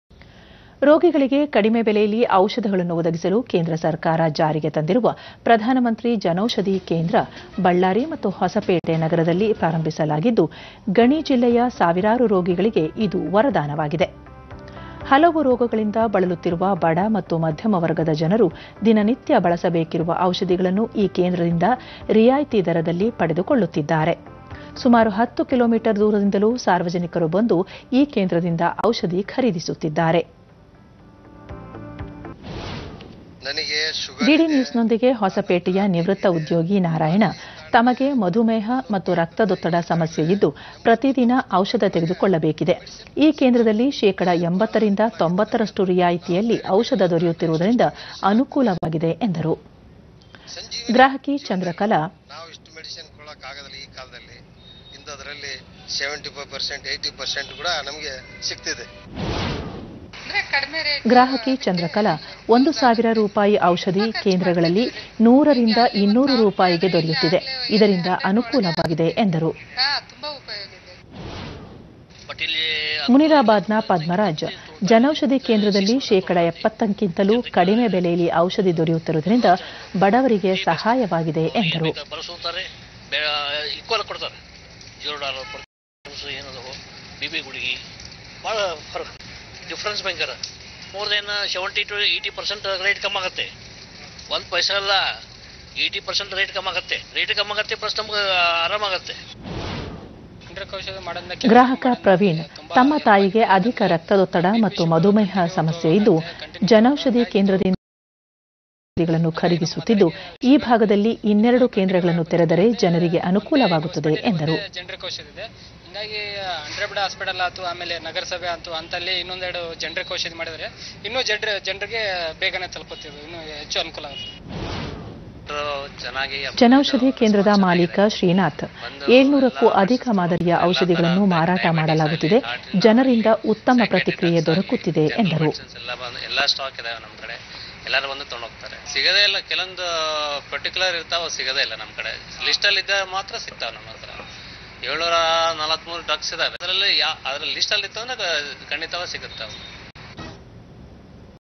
Jan Aushadhi Kendra- Ballary-Ground Report
Jan Aushadhi Kendra in Ballary and Hospet is helping the common man. People come from far off places and are buying medicines at a very low price.They say that their medical expenditure has drastically come down